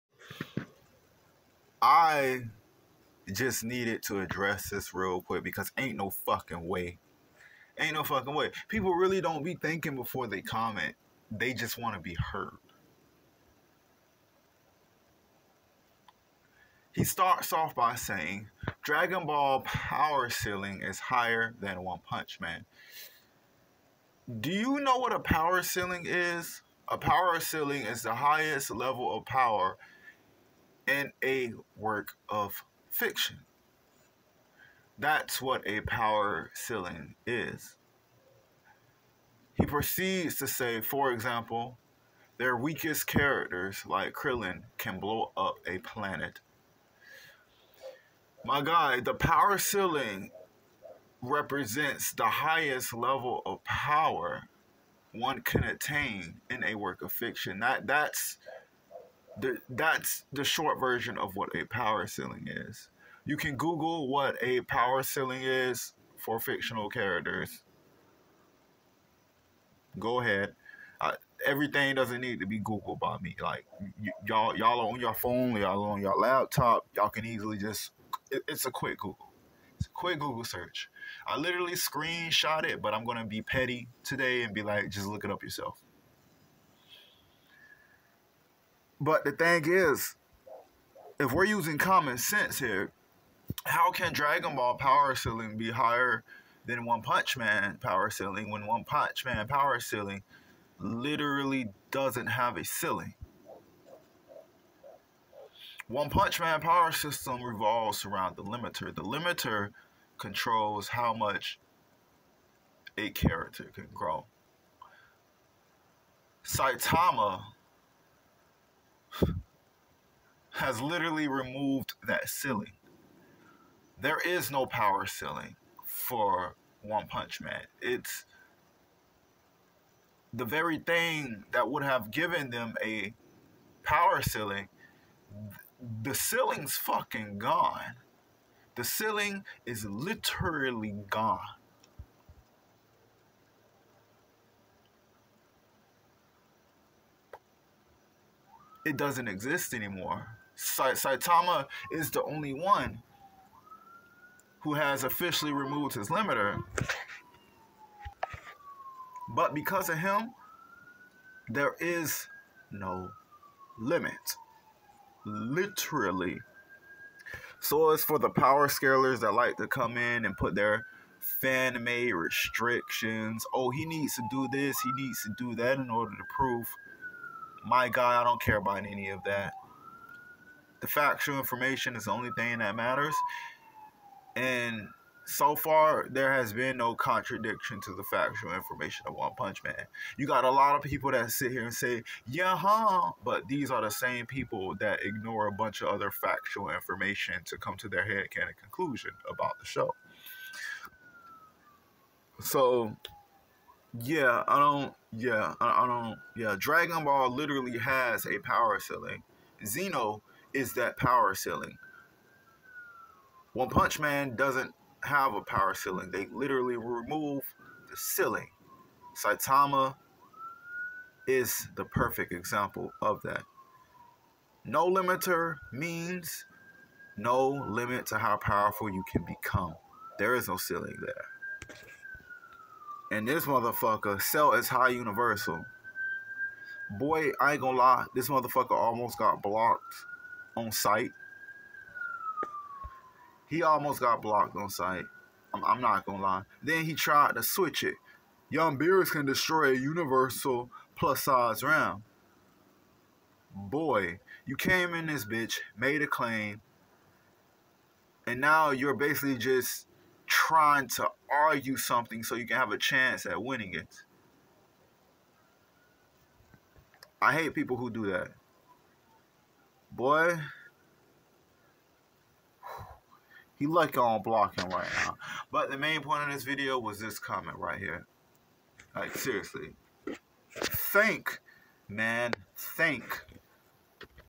Is dragon Ball power ceiling higher than one punch man?
spoiler alert no the dragon Ball franchise power ceiling is not higher than one punch Man's power ceiling.